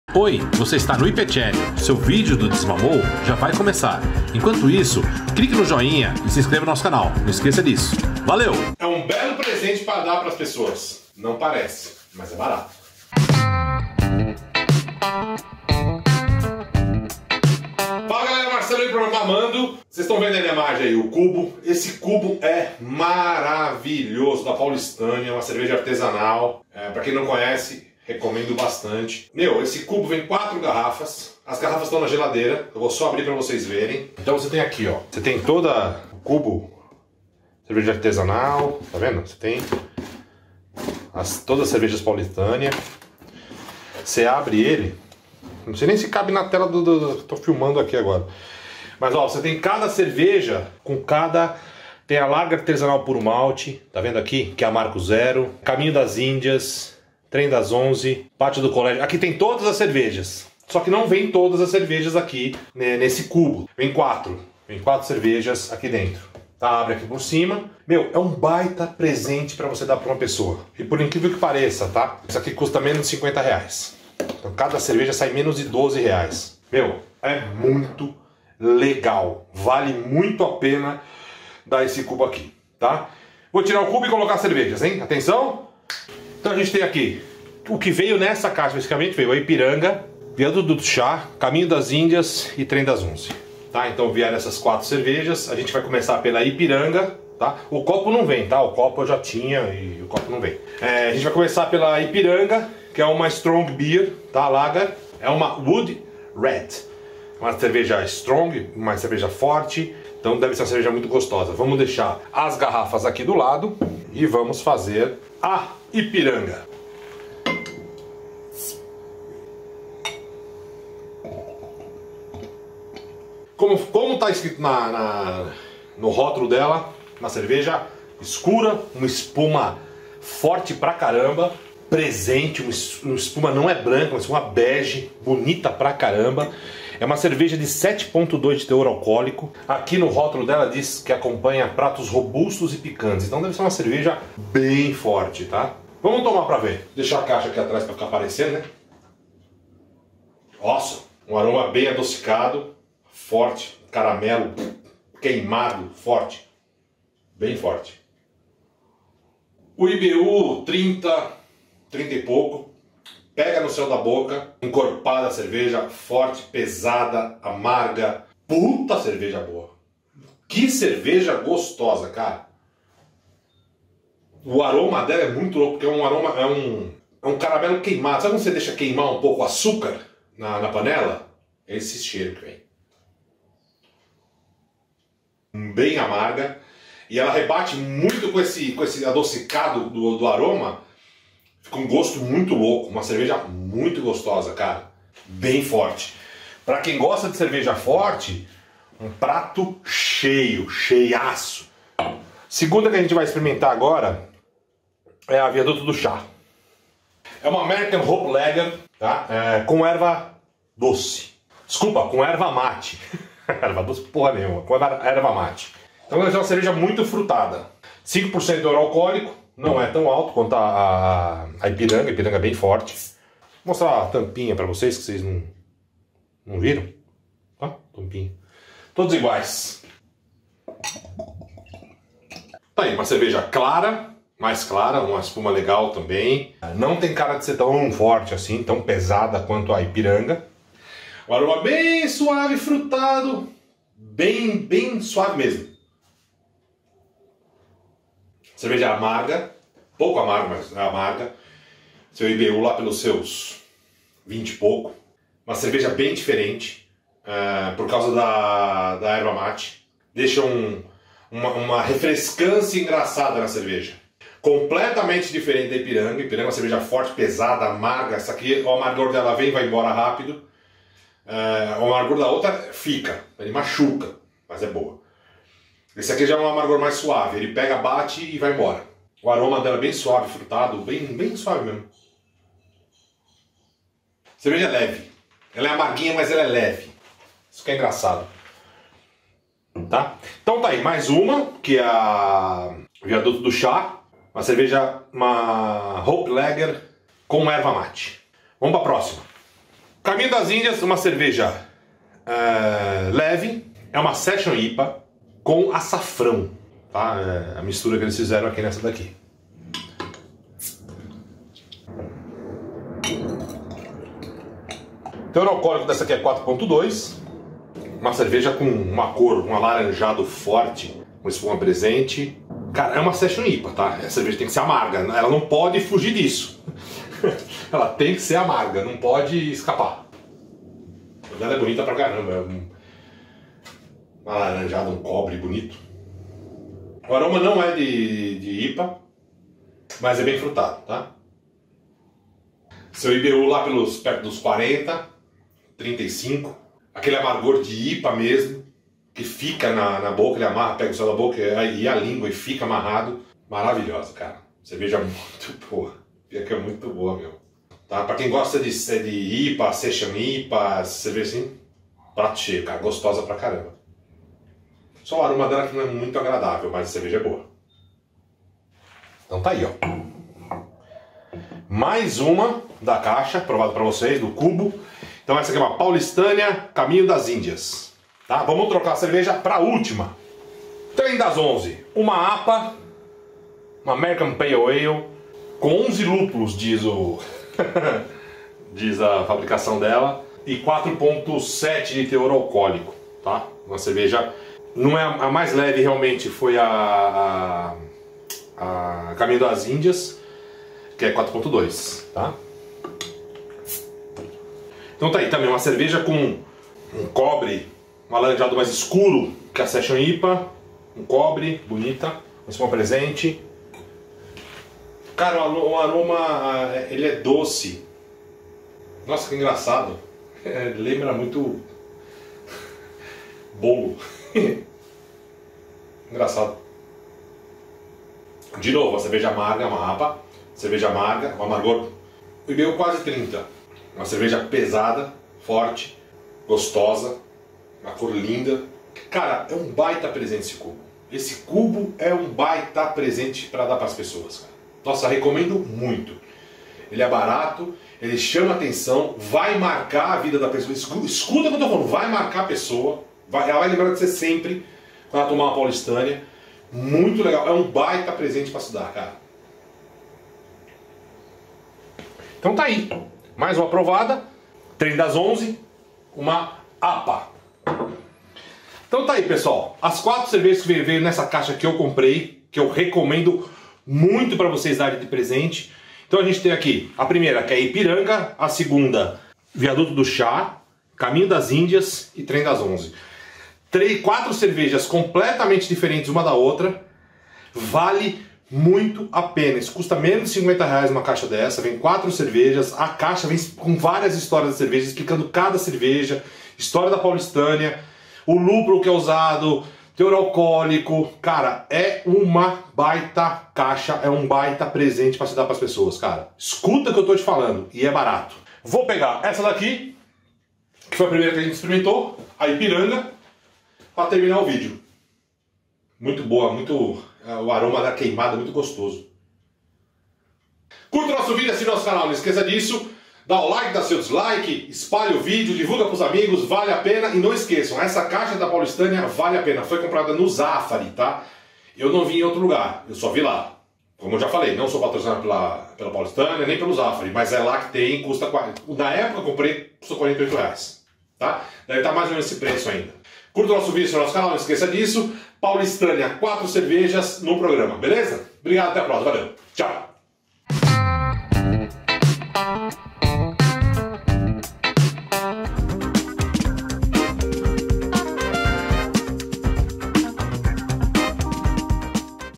MAMANDO | CUBO DA PAULISTÂNIA - UM PRESENTE MARAVILHO E BARATO - VALE A PENA PROCURAR
Este é um Cubo que a Paulistânia lançou e serve como presente. Vale muito a pena você comprar e dar para alguém como comprar para você mesmo. São 4 cerveja diferentes. Assistam o vídeo no YouTube e vejam a qualidade e a degustação de cada cerveja.

Compre relógios, drones e outras coisas importadas, aqui mesmo, no Brasil, acesse